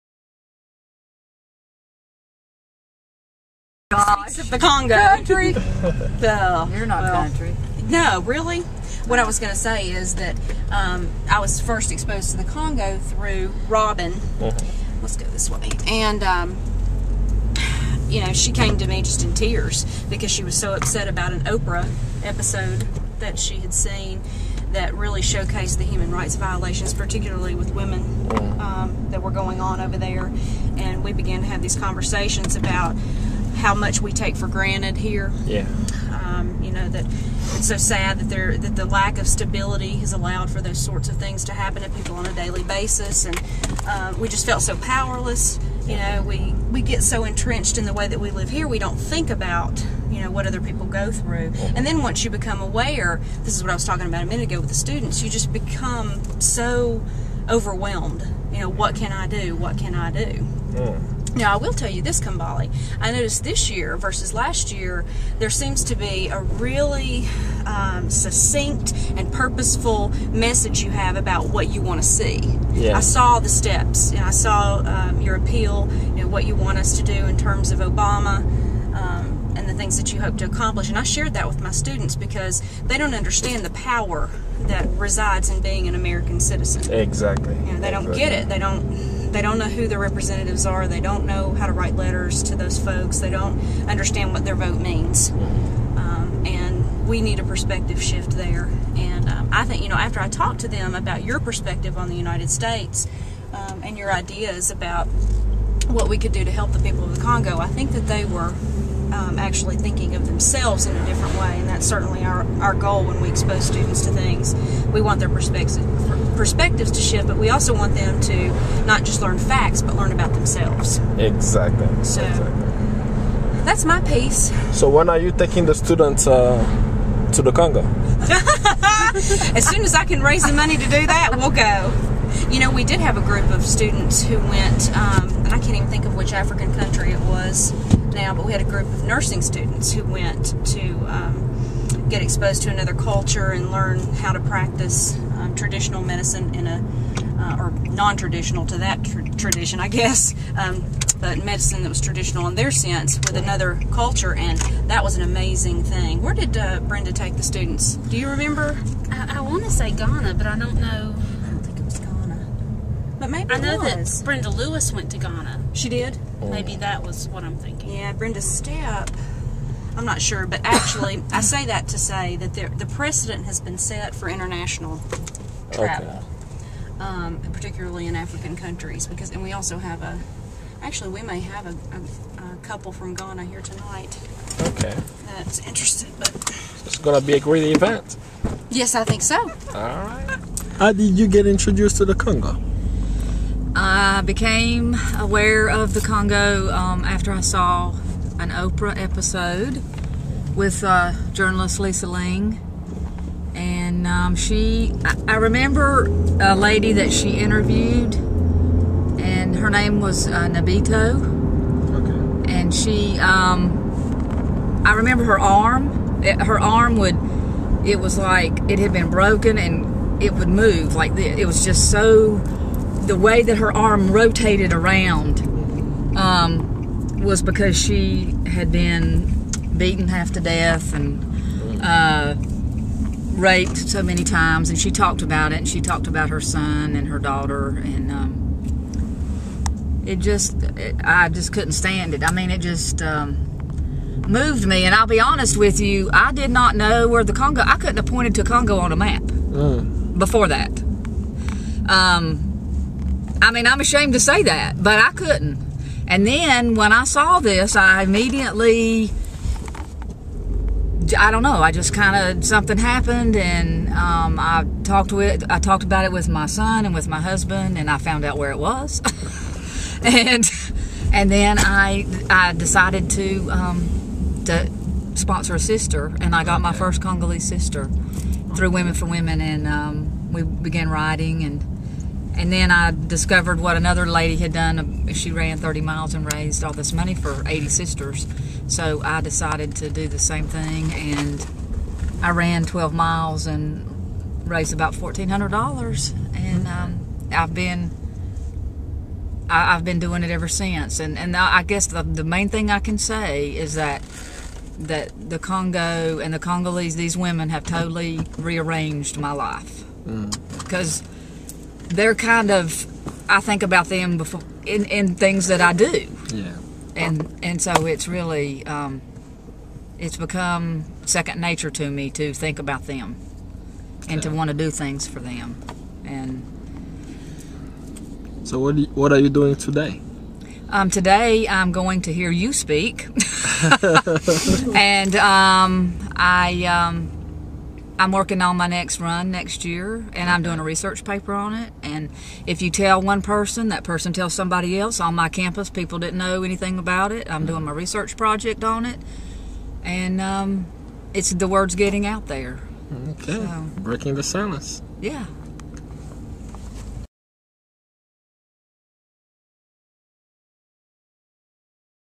Congo Week organizers at Catawba College break the silence
A conversation with the Congo Week organizers about how they were introduced to the Congo cause.